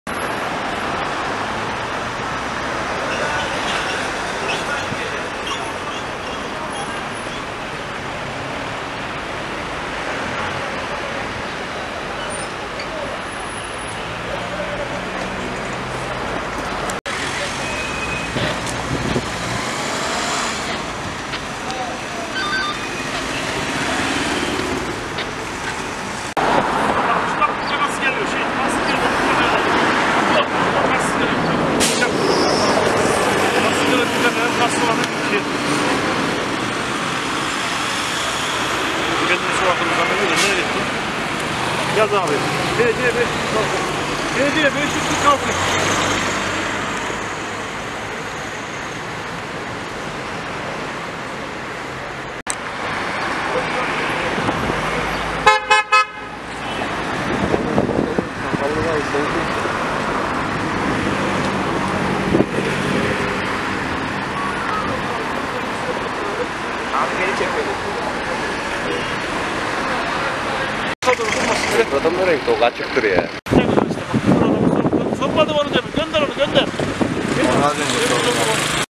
Karaman Haber - Karaman’da Motosikletler Üzerine Trafik Uygulamaları
Karaman'dan Güncel Haberler - Bahar ayının gelmesi havaların ısınması ile Karaman’da motosiklet kullanımı artmaya başladı